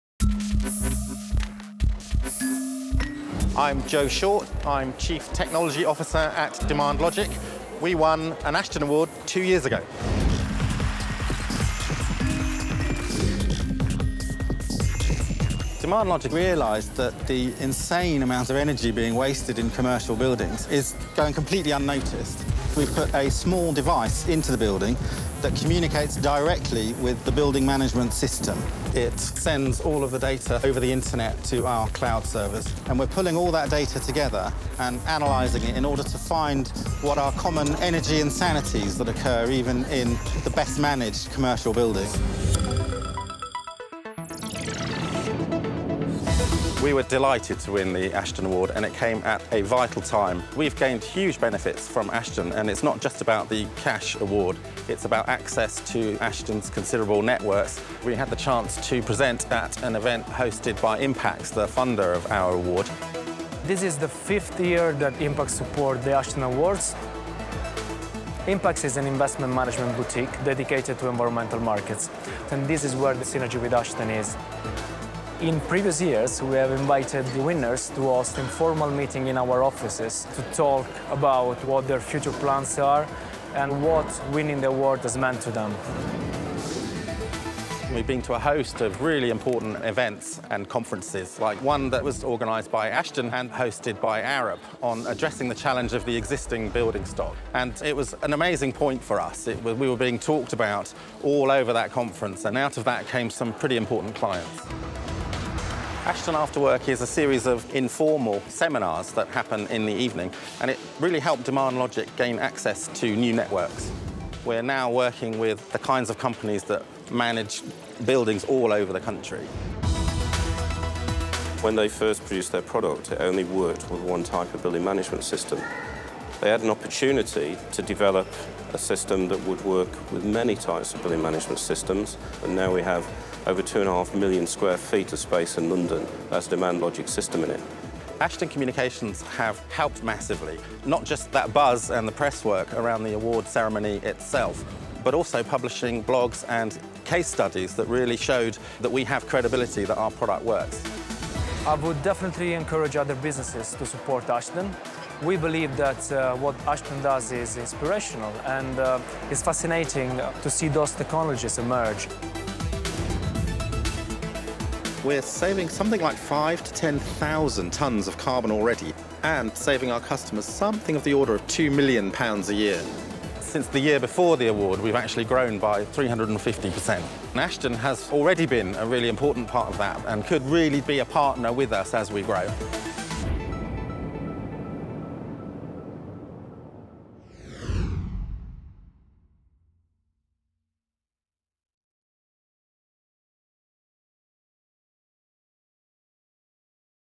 Demand Logic revisited: The impact of winning an Ashden Award
We talk to Demand Logic, a 2015 Ashden Award winner, on progress since the win, how being in the Ashden Alumni network has helped them and what's in store for the future. Impax Asset Management also talk about what it's been like for them, as the Award supporter.